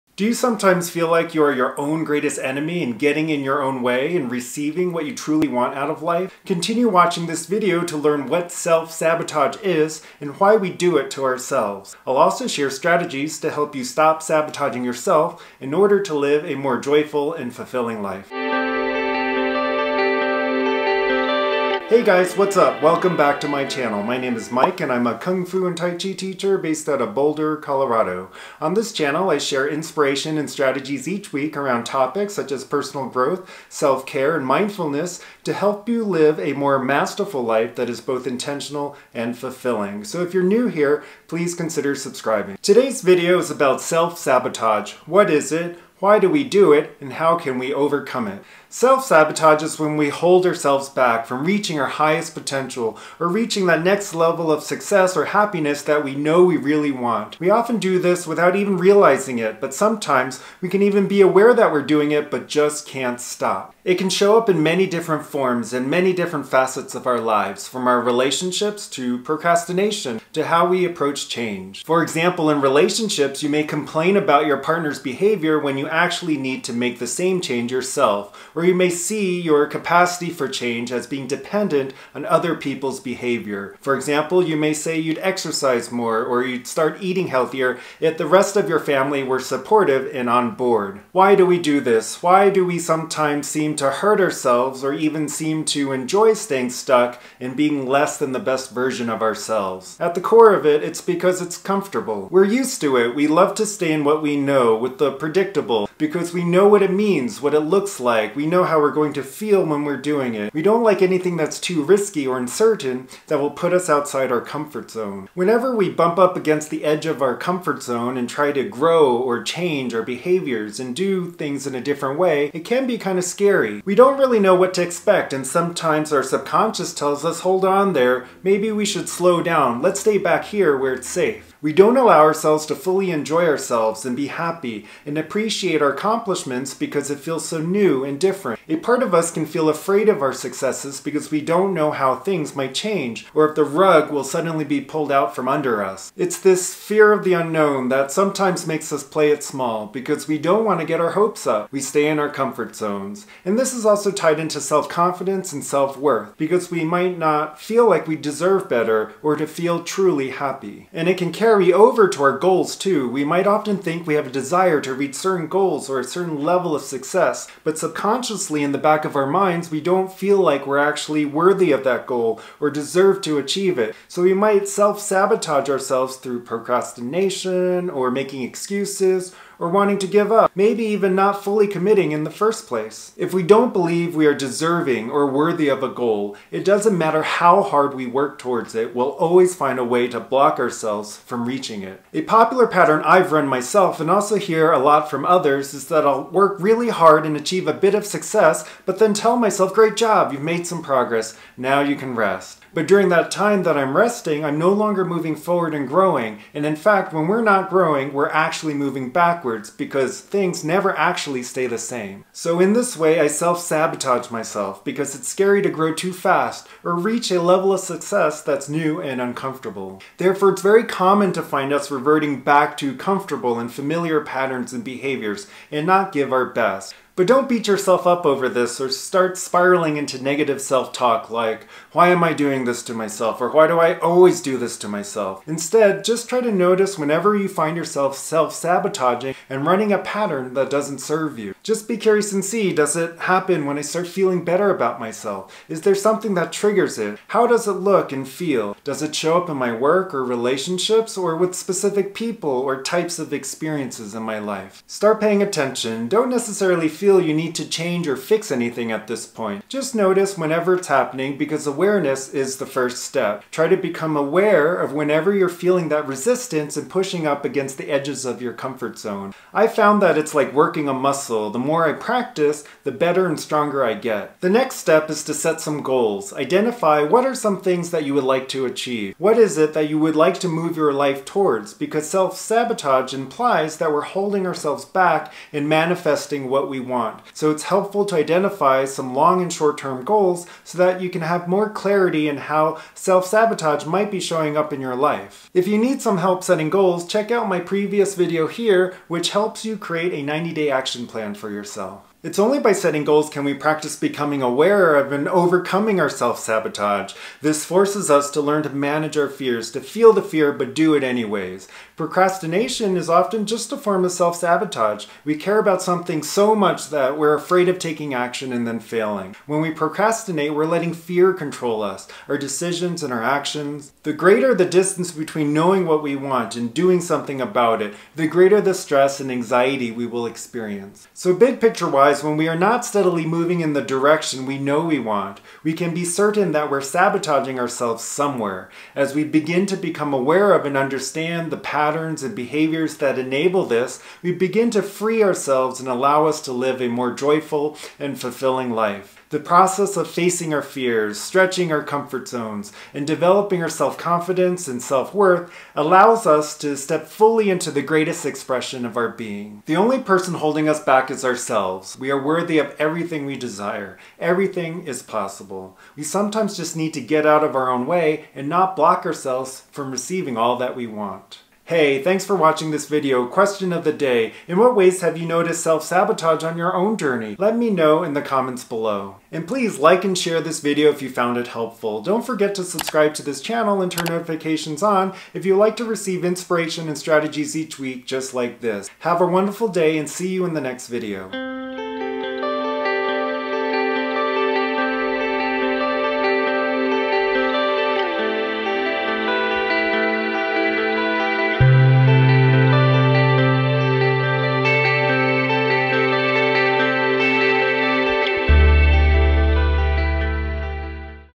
What is Self Sabotage and How to Stop Sabotaging Yourself
Learn what self-sabotage is and how it's blocking you from achieving what you truly want out of life. In this video, I share strategies to help you stop sabotaging yourself to live a more joyful and fulfilling life.

//  N E W  H E R E ?

Snail mail:

Boulder Kung Fu Academy
3200 Carbon Place, Suite 105

In the video, I talk about what self sabotage is, why we do it and how to stop it. Self sabotaging behavior creates resistance and holds us back from achieving what we truly want out of life. It's directly tied to procrastination and our feelings of self-confidence and self-worth. We are often our worst enemy and get in our own way. When there's a gap between knowing what we want and taking action to make it happen, we manifest stress and anxiety into our lives and it hinders our ability to live a joyful and fulfilling life.